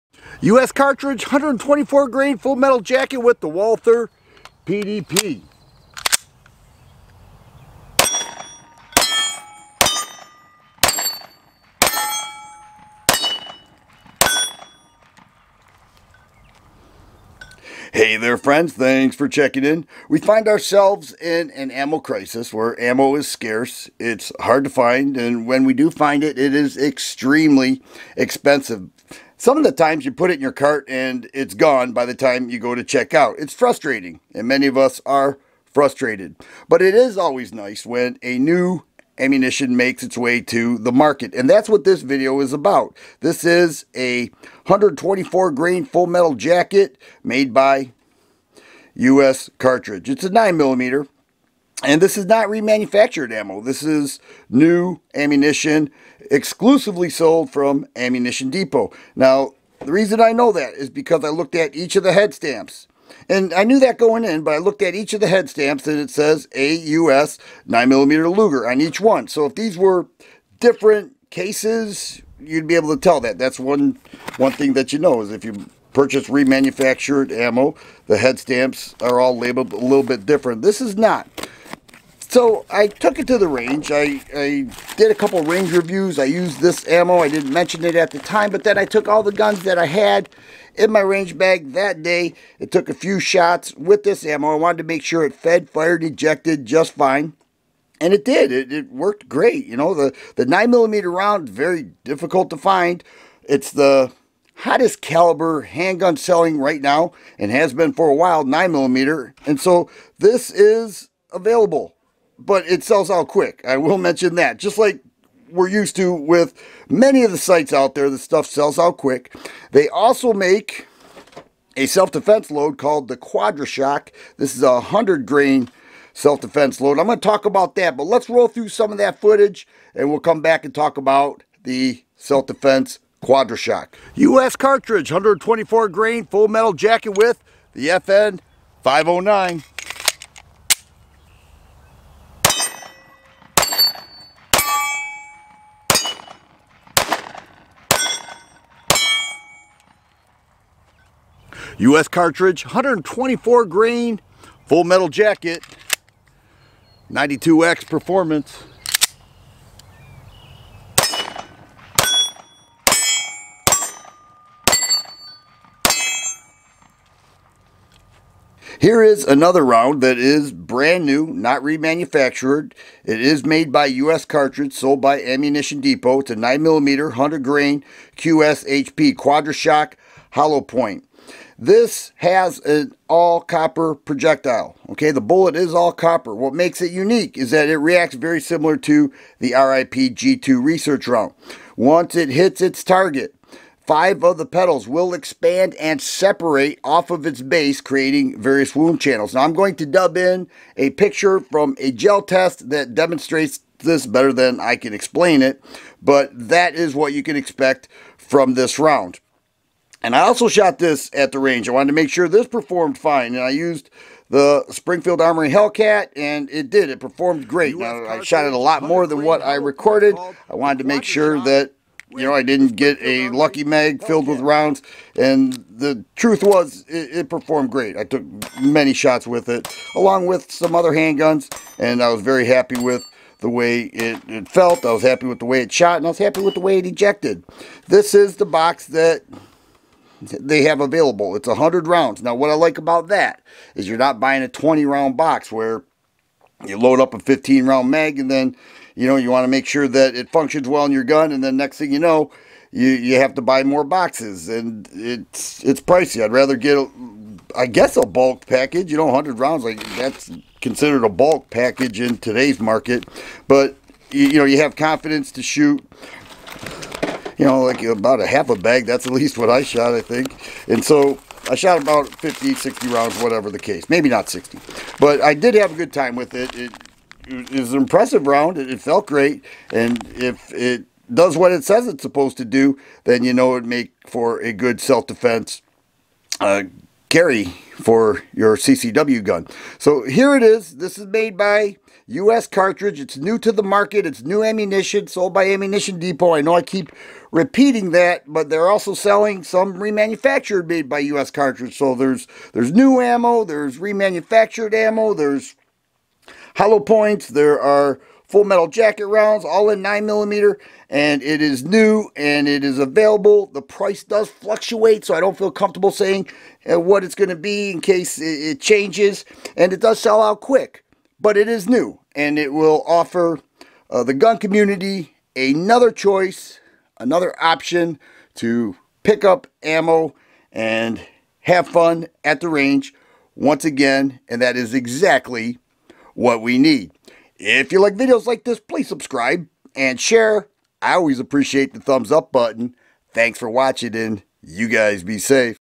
NEW Ammo to the Market "US Cartridge" - TheFirearmGuy
Here is a "New Ammo to the Market called US Cartridge. I tested this ammo at the range and it performed flawlessly. It is available in 9mm 124 grain full metal and 9mm 100 grain self defense loads called "Quadra Shock." Check it out and let me know if you are interested in this new offering.